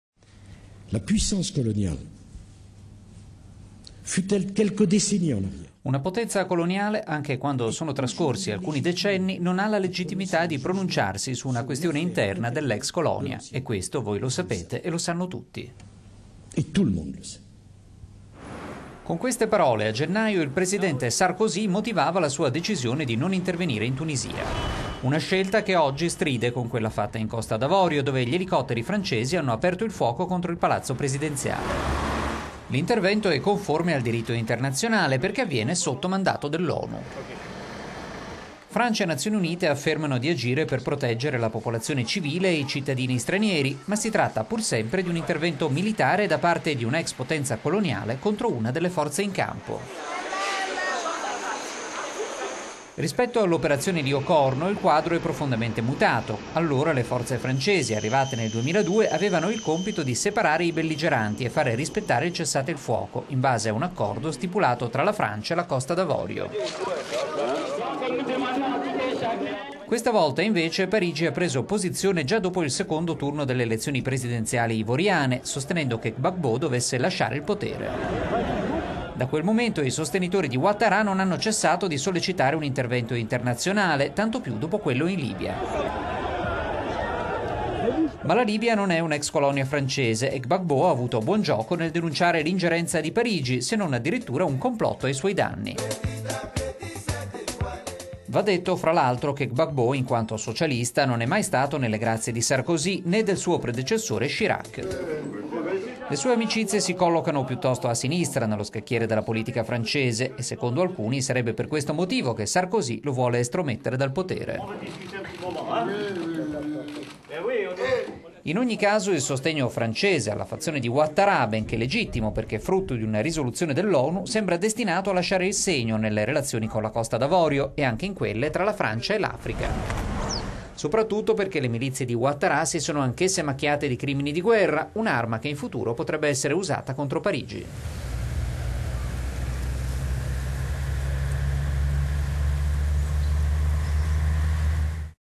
Parigi e l'ambiguità dell'intervento in Costa d'Avorio
Nicolas Sarkozy: "Una potenza coloniale, anche quando sono trascorsi alcuni decenni, non ha la legittimità di pronunciarsi su una questione interna dell'ex colonia. E questo voi lo sapete, e lo sanno tutti".

Con queste parole, a gennaio, il presidente Sarkozy motivava la decisione di non intervenire in Tunisia. Una scelta che, oggi, stride con quella fatta in Costa d'Avorio, dove gli elicotteri francesi hanno aperto il fuoco contro il palazzo presidenziale.